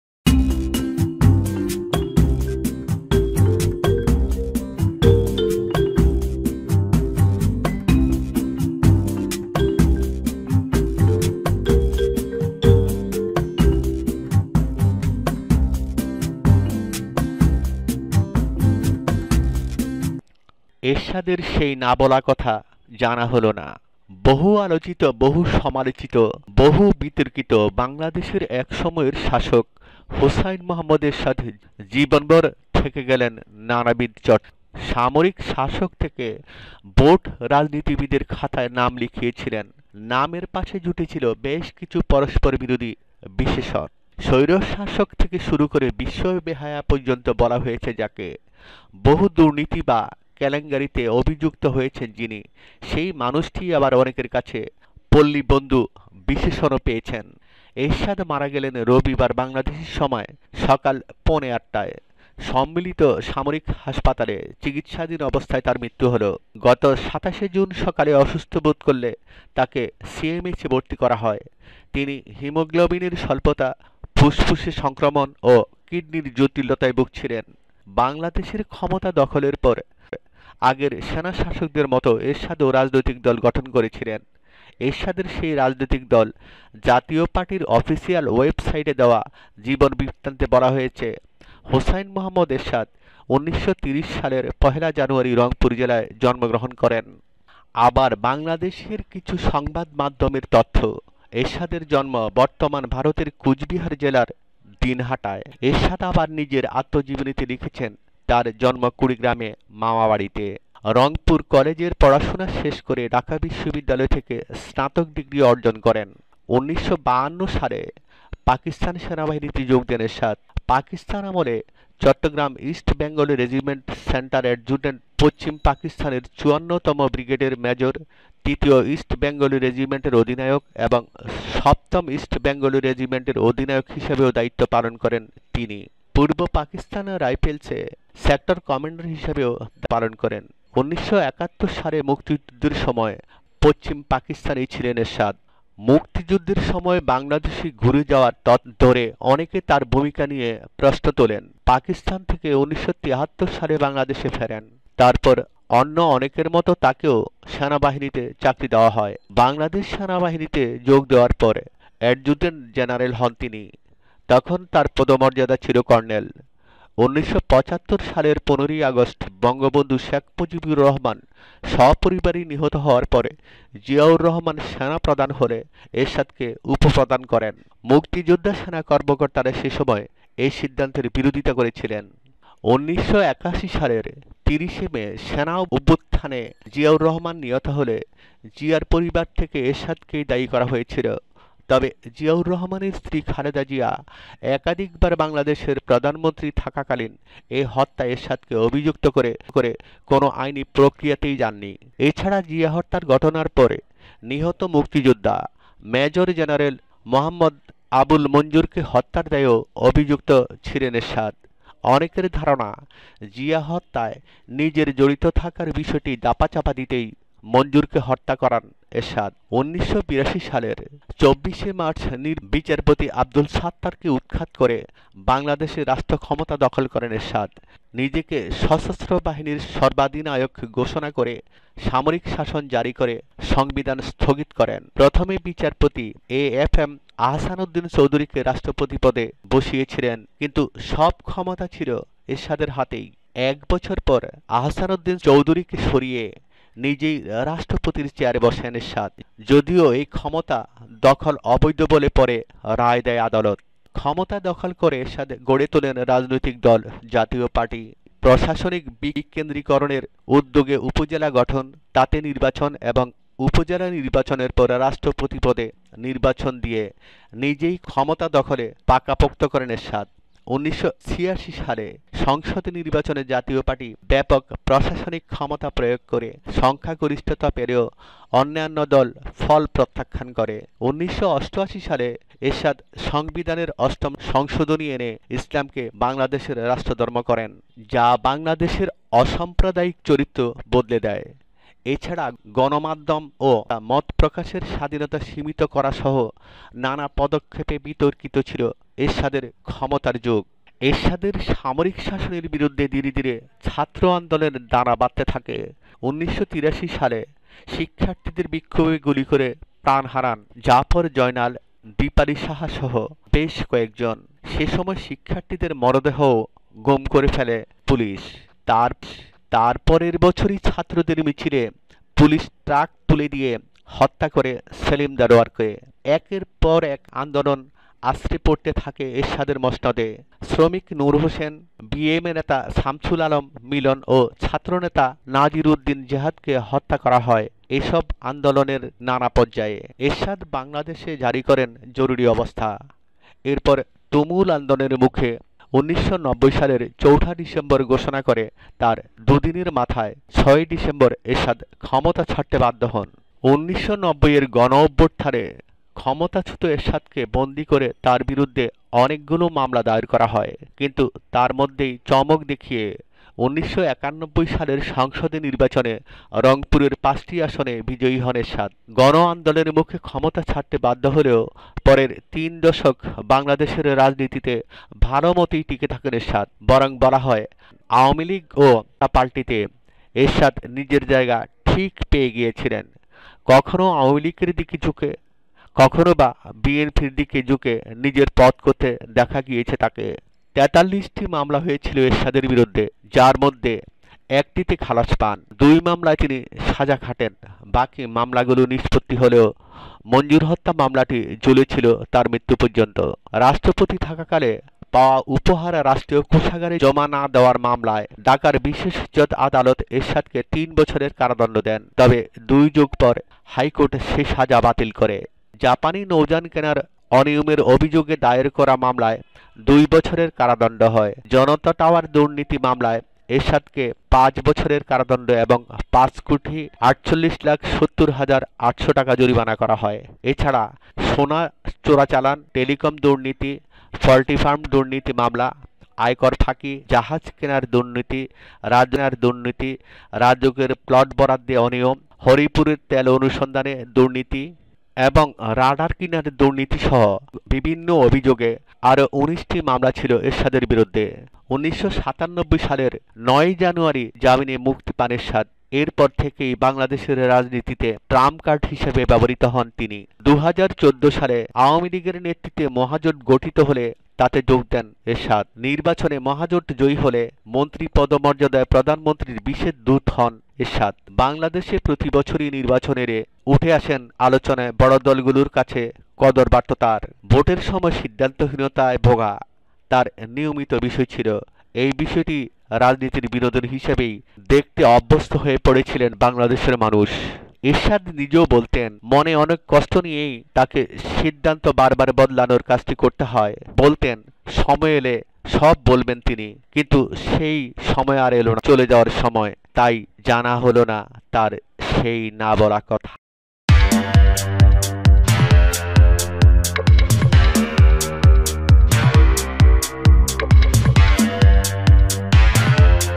এরশাদের সেই না বলা কথা জানা হল না(Ershad did not know what he said)
এরশাদের সেই না বলা কথা জানা হল না
বহু আলোচিত- বহু সমালোচিত- বহু বিতর্কিত... বাংলাদেশের এক সময়ের শাসক হুসেইন মুহম্মদ এরশাদ জীবনভর থেকে গেলেন নানাবিধ চর্চায়। সামরিক শাসক থেকে ভোট-রাজনীতিবিদের খাতায় নাম লিখিয়েছিলেন। নামের পাশে জুটেছিল বেশ কিছু পরস্পর বিরোধী বিশেষণ। ‘স্বৈরশাসক’ থেকে শুরু করে ‘বিশ্ব বেহায়া’ পর্যন্ত বলা হয়েছে যাঁকে, বহু দুর্নীতি বা নারী কেলেঙ্কারীতে অভিযুক্ত হয়েছেন যিনি, সেই মানুষটিই আবার অনেকের কাছে ‘পল্লিবন্ধু’ বিশেষণও পেয়েছেন।
এরশাদ মারা গেলেন রবিবার বাংলাদেশি সময় সকাল পৌনে ৮টায়। ঢাকার সম্মিলিত সামরিক হাসপাতাল (সিএমএইচ)-এ চিকিৎসাধীন অবস্থায় তাঁর মৃত্যু হল। গত ২৭ জুন সকালে অসুস্থবোধ করলে তাঁকে সিএমএইচে ভর্তি করা হয়। তিনি হিমোগ্লোবিনের স্বল্পতা, ফুসফুসে সংক্রমণ ও কিডনির জটিলতায় ভুগছিলেন।
বাংলাদেশের রাষ্ট্রক্ষমতা দখলের পর, আগের সেনাশাসকের মতো এরশাদও রাজনৈতিক দল গঠন করেছিলেন। এরশাদের সেই রাজনৈতিক দল জাতীয় পার্টির অফিসিয়াল ওয়েবসাইটে দেওয়া জীবনবৃত্তান্তে বলা হয়েছে, হুসেইন মুহম্মদ এরশাদ ১৯৩০ সালের ১ ফেব্রুয়ারি রংপুর জেলায় জন্মগ্রহণ করেন। আবার বাংলাদেশের কিছু সংবাদমাধ্যমের তথ্য, এরশাদের জন্ম বর্তমান ভারতের কোচবিহার জেলার দিনহাটায়। এরশাদ আবার নিজের আত্মজীবনীতে লিখেছেন, তাঁর জন্ম কুড়িগ্রামে মামা বাড়িতে। রংপুরে কলেজের পড়াশোনা শেষ করে ঢাকা বিশ্ববিদ্যালয় থেকে স্নাতক ডিগ্রি অর্জন করেন।
১৯৫২ সালে পাকিস্তান সেনাবাহিনীতে যোগ দেন এরশাদ। পাকিস্তান আমলে চট্টগ্রাম ইস্টবেঙ্গল রেজিমেন্টাল সেন্টারে অ্যাডজুট্যান্ট, পশ্চিম পাকিস্তানের ৫৪তম ব্রিগেডের মেজর, তৃতীয় ইস্টবেঙ্গল রেজিমেন্টে অধিনায়ক এবং সপ্তম ইস্টবেঙ্গল রেজিমেন্টের অধিনায়ক হিসেবেও দায়িত্ব পালন করেন তিনি। পূর্ব পাকিস্তান রাইফেলস (ইপিআর)-এ সেক্টর কমান্ডার হিসেবেও দায়িত্ব পালন করেন। ১৯৭১ সালে মুক্তিযুদ্ধের সময় পশ্চিম পাকিস্তানেই ছিলেন এরশাদ। মুক্তিযুদ্ধের সময় বাংলাদেশ ঘুরে যাওয়ার তত্য তুলে ধরে অনেকে তাঁর ভূমিকা নিয়ে প্রশ্ন তোলেন। পাকিস্তান থেকে ১৯৭৩ সালে বাংলাদেশে ফেরেন। তার পর অন্য অনেকের মত তাঁকেও সেনাবাহিনীর চাকরিতে নেওয়া হয়।
বাংলাদেশ সেনাবাহিনীতে যোগ দেওয়ার পর অ্যাডজুট্যান্ট জেনারেল হন তিনি। তখন তাঁর পদমর্যাদা ছিল কর্নেল। ১৯৭৫ সালের ১৫ অগস্ট বঙ্গবন্ধু শেখ মুজিবুর রহমান সপরিবারে নিহত হওয়ার পর, জিয়াউর রহমান সেনাপ্রধান হলে, এরশাদকে উপপ্রধান করেন। মুক্তিযোদ্ধা সেনা কর্তারা সে সময় এই সিদ্ধান্তের বিরোধিতা করেছিলেন।
১৯৮১ সালের ৩০ মে সেনা অভ্যুত্থানে জিয়াউর রহমান নিহত হলে, জিয়ার পরিবার থেকে এরাশাদকেই দায়ী করা হয়েছিল। তবে জিয়াউর রহমানের স্ত্রী খালেদা জিয়া একাধিকবার বাংলাদেশের প্রধানমন্ত্রী থাকাকালীন, এ হত্যায় এরশাদকে অভিযুক্ত করে কোনও আইনি প্রক্রিয়াতেই যাননি। এ ছাড়া জিয়া হত্যার ঘটনার পর, নিহত মুক্তিযোদ্ধা মেজর জেনারেল মহম্মদ আবুল মঞ্জুরকে হত্যার দায়েও অভিযুক্ত ছিলেন এরশাদ। অনেকের ধারনা, জিয়া হত্যায় নিজের জড়িত থাকার বিষয়টি ধামাচাপা দিতেই মঞ্জুরকে হত্যা করান এরশাদ।
১৯৮২ সালের ২৪ মার্চ নির্বাচিত রাষ্ট্রপতি বিচারপতি আবদুস সাত্তারকে উৎখাত করে বাংলাদেশে রাষ্ট্রক্ষমতা দখল করেন এরশাদ। নিজেকে সশস্ত্রবাহিনীর সর্বাধিনায়ক ঘোষণা করে সামরিক শাসন জারি করে সংবিধান স্থগিত করেন। প্রথমে বিচারপতি এএফএম আহসানউদ্দিন চৌধুরীকে রাষ্ট্রপতির পদে বসিয়েছিলেন। কিন্তু সব ক্ষমতা ছিল এরশাদেরই হাতে। এক বছর পর আহসানউদ্দিন চৌধুরীকে সরিয়ে নিজেই রাষ্ট্রপতির চেয়ারে বসেন এরশাদ, যদিও এই ক্ষমতা দখল অবৈধ বলে পরে রায়  দেয় আদালত। ক্ষমতা দখল করে এরশাদ গড়ে তোলেন রাজনৈতিক দল ‘জাতীয় পার্টি’। প্রশাসনিক বিকেন্দ্রীকরণের উদ্যোগে উপজেলা গঠন, তাতে নির্বাচন এবং উপজেলা নির্বাচনের পর রাষ্ট্রপতি পদে নির্বাচন দিয়ে নিজের ক্ষমতা দখলকে পোক্ত করেন এরশাদ। এর পর ১৯৮৬ সালে সংসদ নির্বাচনে জাতীয় পার্টি ব্যাপক প্রশাসনিক ক্ষমতা প্রয়োগ করে সংখ্যাগরিষ্ঠতা পেলেও, অন্যান্য দল ফলাফল প্রত্যাখ্যান করে। ১৯৮৮ সালে এরশাদ সংবিধানের অষ্টম সংশোধনী এনে ইসলামকে বাংলাদেশের রাষ্ট্রধর্ম  করেন, যা বাংলাদেশের অসাম্প্রদায়িক চরিত্র বদলে দেয়। এ ছাড়া গণমাধ্যম ও মত প্রকাশের স্বাধীনতা সীমিত করা-সহ নানা পদক্ষেপে বিতর্কিত ছিল এরশাদের ক্ষমতার যুগ।
এরশাদের সামরিক শাসনের বিরুদ্ধে ধীরে ধীরে ছাত্র আন্দোলন দানা বাঁধতে থাকে। ১৯৮৩ সালে সালে শিক্ষার্থীদের বিক্ষোভে গুলিতে প্রাণ হারান জাফর, জয়নাল, দীপালী সাহা-সহ বেশ কয়েকজন। সে সময় শিক্ষার্থীদের মরদেহও গুম করে ফেলে পুলিশ। তার পরের বছরই ছাত্রদের মিছিলে পুলিশ ট্রাক তুলে দিয়ে হত্যা করে সেলিম-দেলোয়ারকে। একের পর এক আন্দোলন আছড়ে পড়তে থাকে এরশাদের মসনদে। শ্রমিক নুর হোসেন, বিএমএ নেতা শামসুল আলম মিলন ও ছাত্রনেতা নাজির উদ্দিন জেহাদকে হত্যা করা হয় এ সব আন্দোলনের নানা পর্যায়ে। এরশাদ বাংলাদেশে জারি করেন জরুরি অবস্থা। এর পর তুমুল আন্দোলনের মুখে, ১৯৯০ সালের ৪ ডিসেম্বর ঘোষণা করে, তার দু’দিনের মাথায় ৬ ডিসেম্বর এরশাদ ক্ষমতা ছাড়তে বাধ্য হন।
১৯৯০-এর গণ আন্দোলনে ক্ষমতাচ্যুত এরশাদকে বন্দি করে তার বিরুদ্ধে অনেকগুলো মামলা দায়ের করা হয়। কিন্তু তার মধ্যেই চমক দেখিয়ে ১৯৯১ সালের সংসদ নির্বাচনে রংপুরের ৫টি আসনে বিজয়ী হন এরশাদ। গণ আন্দোলনের মুখে ক্ষমতা ছাড়তে বাধ্য হলেও, পরের তিন দশক বাংলাদেশের রাজনীতিতে ভাল মতোই টিকে থাকেন এরশাদ। বরং বলা যায়, আওয়ামি লিগ ও বিএনপির পাল্টাপাল্টিতে এরশাদ নিজের জায়গা ঠিক পেয়ে গিয়েছিলেন। কখনও আওয়ামি লিগের দিকে ঝুঁকে, কখনও বা বিএনপির দিকে ঝুঁকে নিজের পথ করতে দেখা গিয়েছে তাঁকে।
৪৩টি মামলা হয়েছিল এরশাদের বিরুদ্ধে। যার মধ্যে একটিতে খালাস পান। দুই মামলায় তিনি সাজা খাটেন। বাকি মামলাগুলোর নিষ্পত্তি হলেও মঞ্জুর হত্যা মামলাটি ঝুলে ছিল তাঁর মৃত্যু পর্যন্ত।